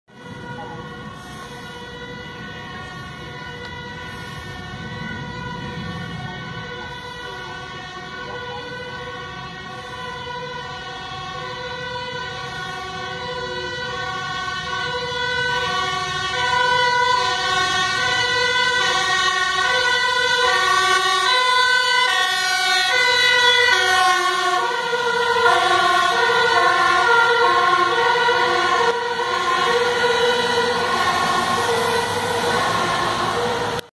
P1 brandweer met spoed naar buitenbrand 14-1241. [#1353]
Brandweer Bussum is met spoed onderweg naar een melding van een buitenbrand in Naarden vesting.